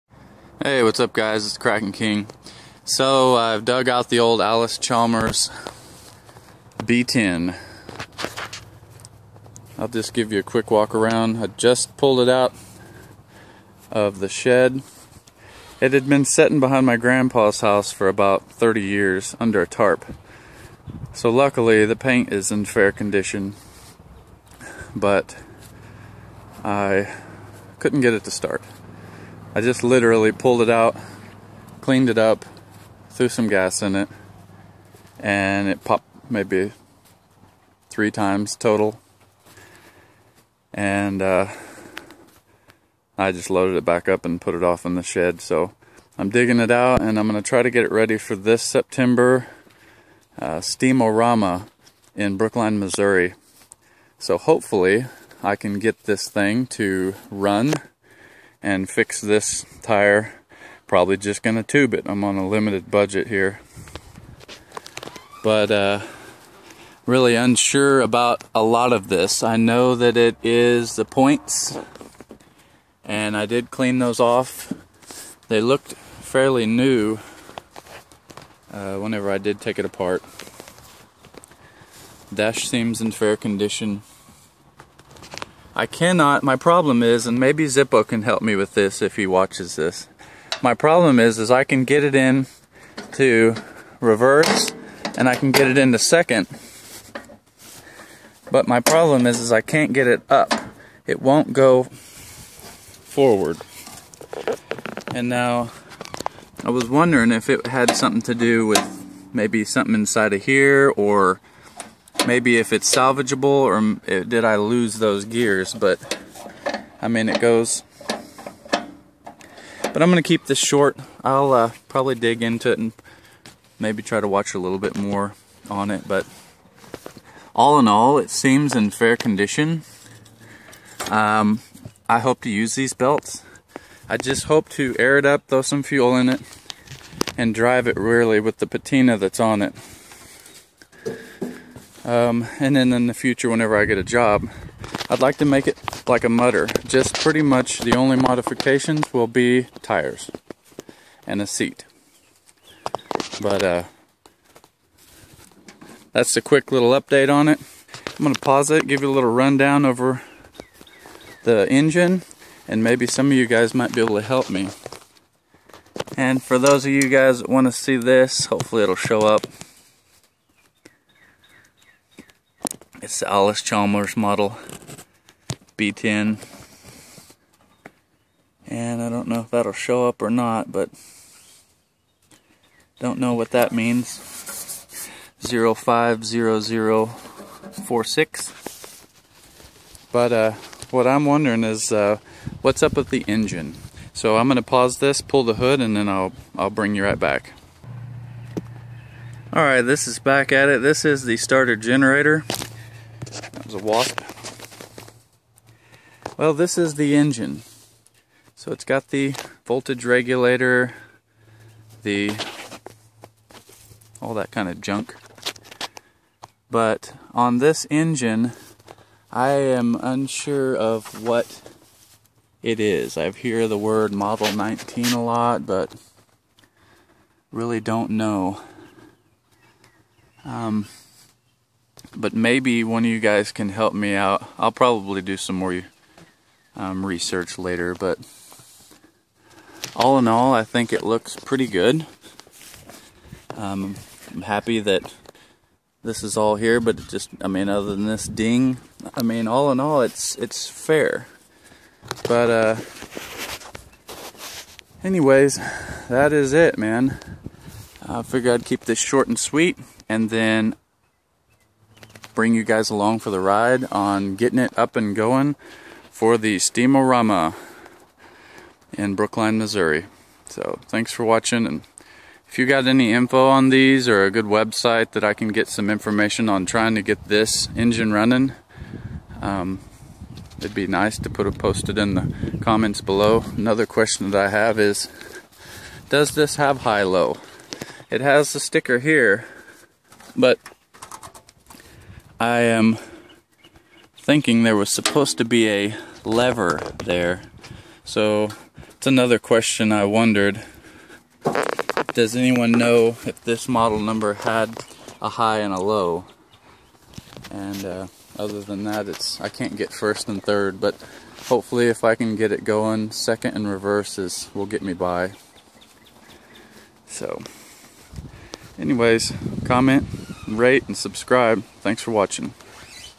Dug out the old Allis Chalmers B10
Just getting the b10 in my shop so i can try to get it up and running for the september Steam-o-ramma tractor and steam engine show in brookine MO, I so want to repower it with a v twin.